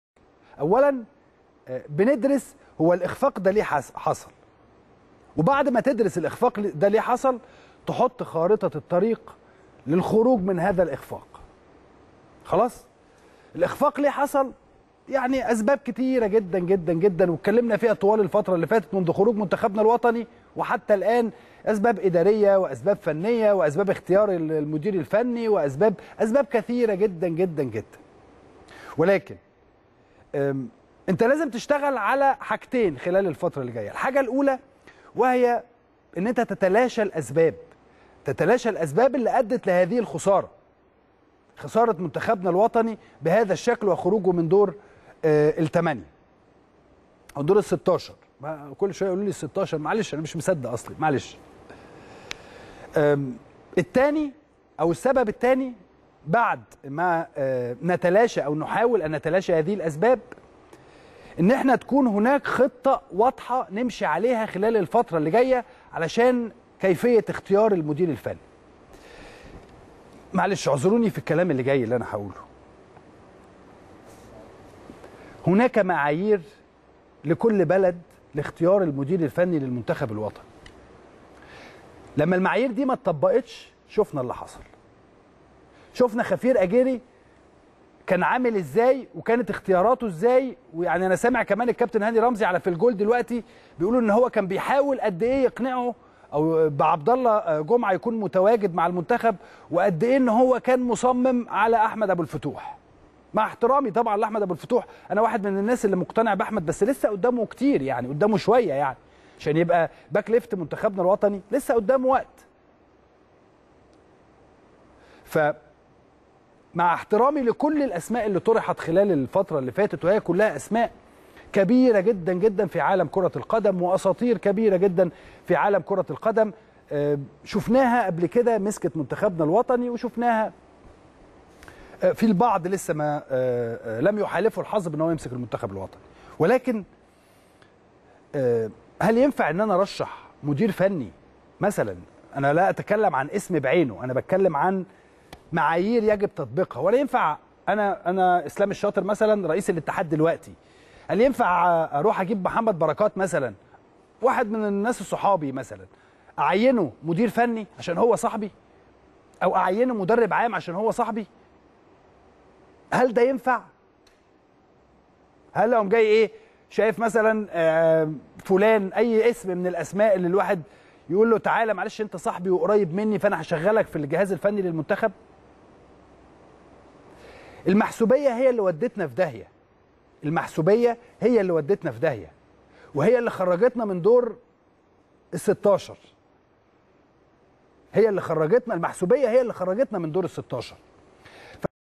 اسلام الشاطر على الهواء : خرجنا من بطولة افريقيا علشان "المحسوبية"
التردد: 12341 أفقي.
القمر: نايل سات.
معامل الخطأ: 5/6.
معدل الترميز: 27500